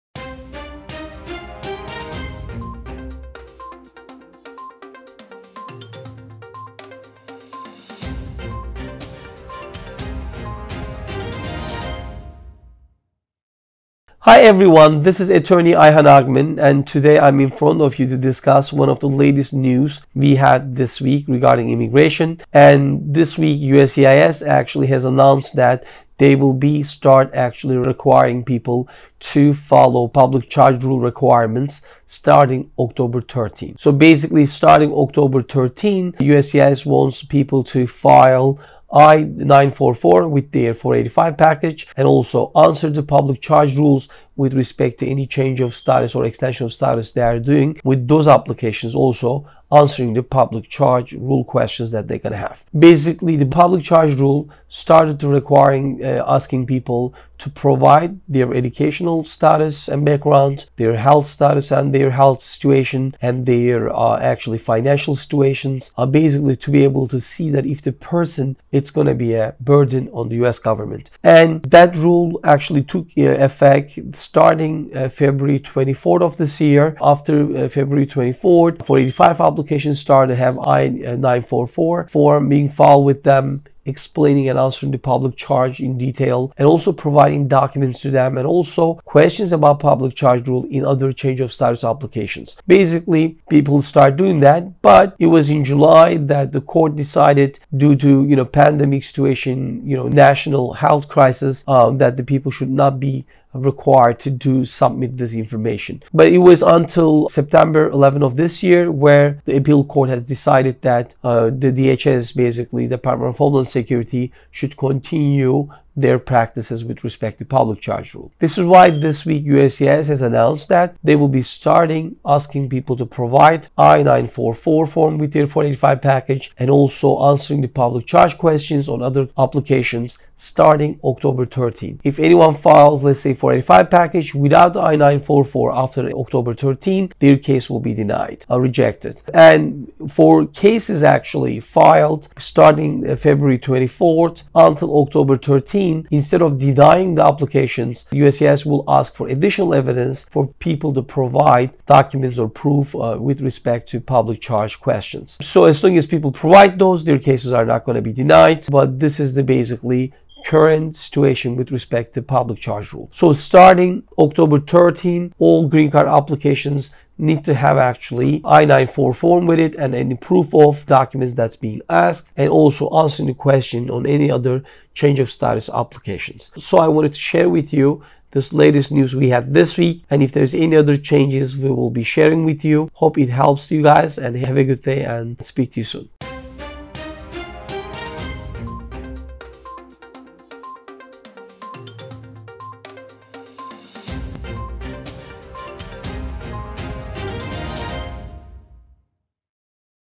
BREAKING NEWS - USCIS REIMPOSES PUBLIC CHARGE RULE FOLLOWING COURT ORDERS !
HOW AND WHEN US VISA OWNERS, GREEN CARD OWNERS AND US CITIZENSHIP APPLICANTS WILL BE AFFECTED FOR PUBLIC CHARGE RULE ?